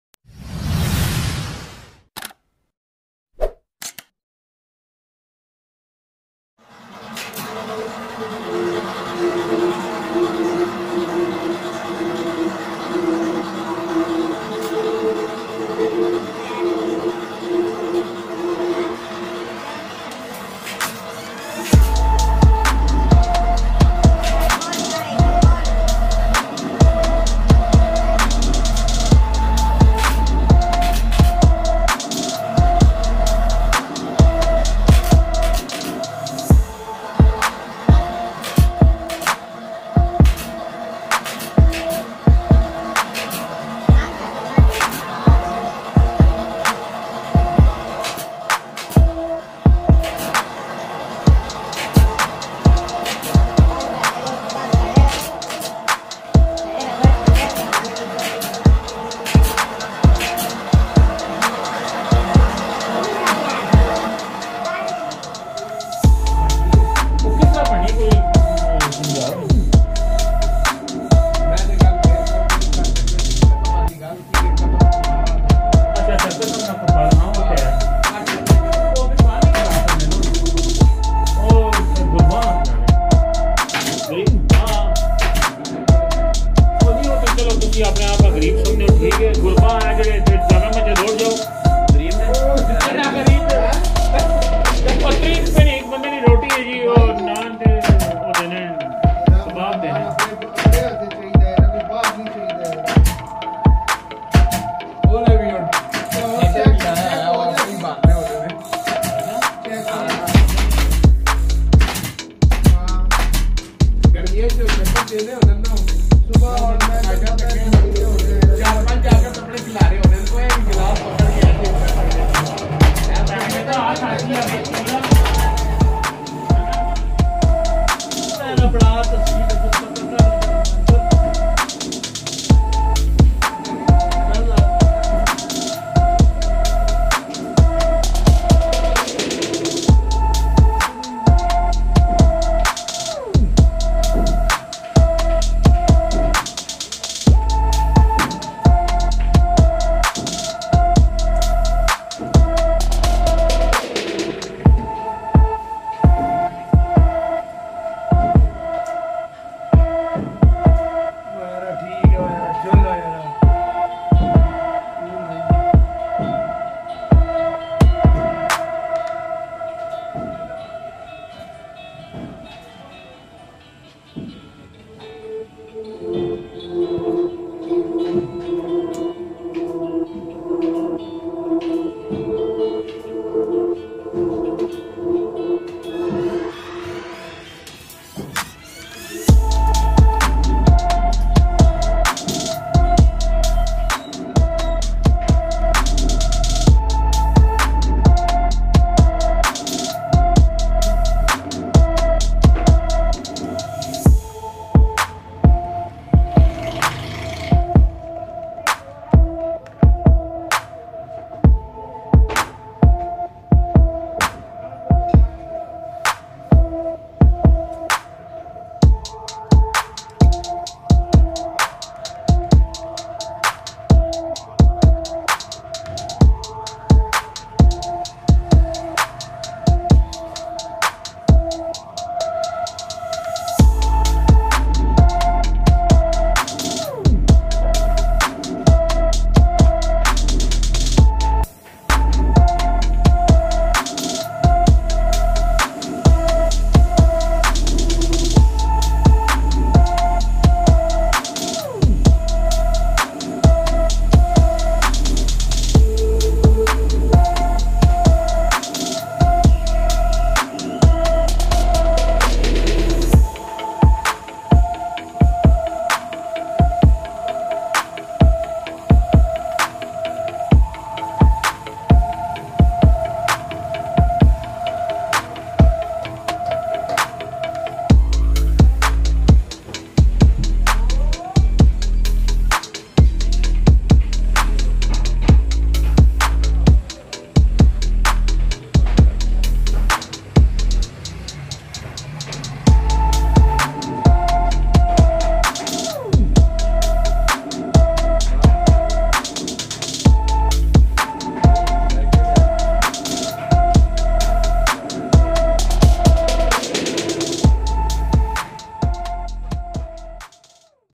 chinioti solid wood frame poshish in workshop rawalpindu
chinioti solid wooden stacture polish and
/full molty foam polish /best sofa /best poshish/and best finshing
rawalpindi pakistan/
my home furnture tv()
youtube chanel